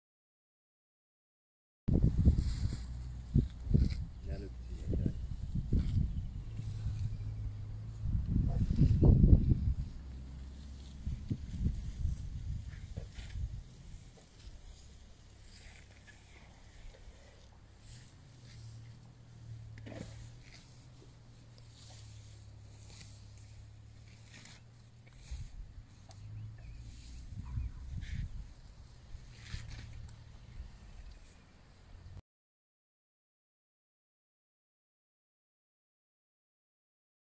Herd of elephants at Sabi Sabi
A small herd of elephants were enjoying the abundance of food around after the recent rains received on the reserve.